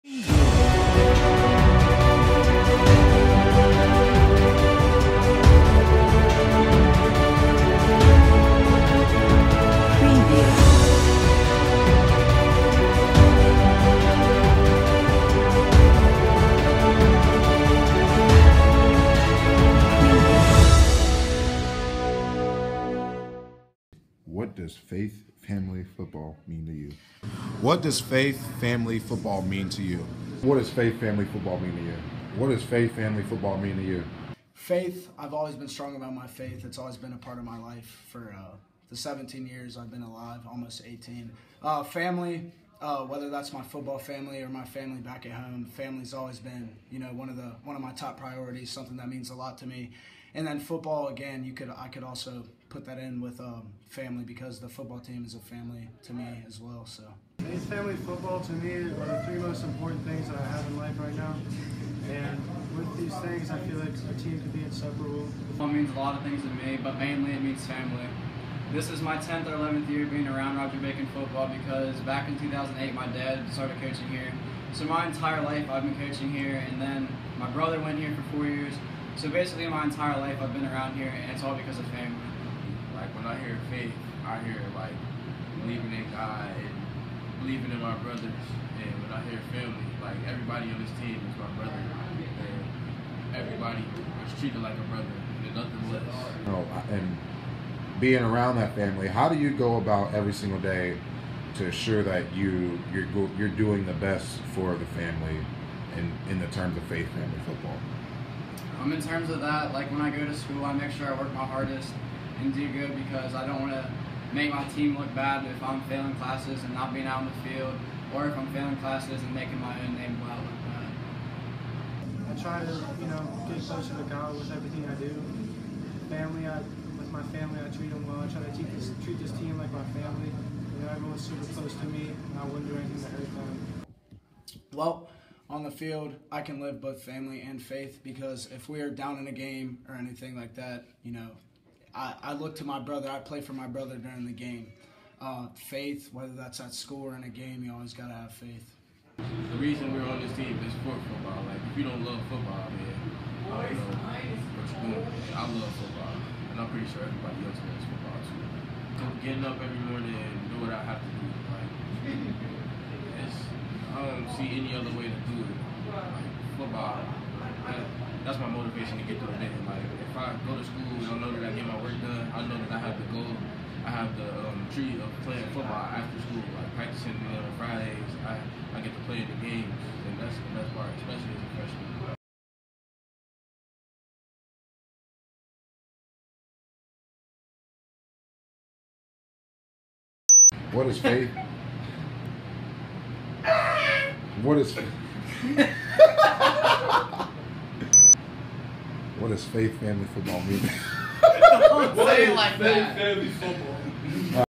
A Look Inside: Faith Family Football
Taking a brief look at what Faith, Family, Football means to our players.